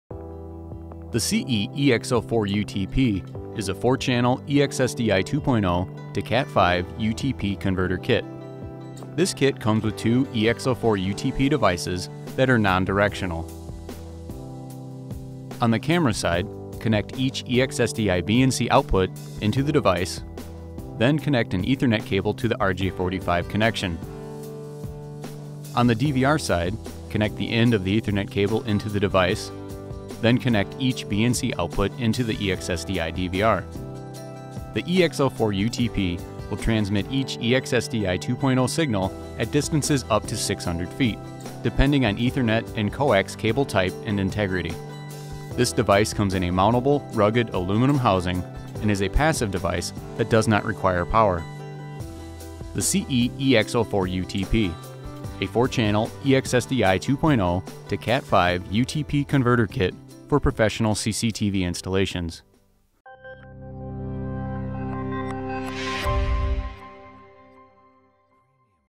CE-EX04UTP: 4ch EX-SDI 2.0 to Cat5/UTP Converter Kit
The CE-EX04UTP is a four channel EX-SDI 2.0 to CAT5 UTP Converter kit.

This kit comes with two EX04UTP devices that are non-directional. (Each unit can be used as a transmitter or receiver).

On the camera side— connect each EX-SDI BNC output into the device, then connect an ethernet cable to the RJ45 connection.

On the DVR side— connect the end of the ethernet cable into the device, then connect each BNC output into the EX-SDI DVR.

The EX04UTP will transmit each EX-SDI 2.0 signal at distances up to 600 ft, depending on ethernet and coax cable type & integrity.

This device comes in a mountable, rugged aluminum housing and is a passive device that does not require power.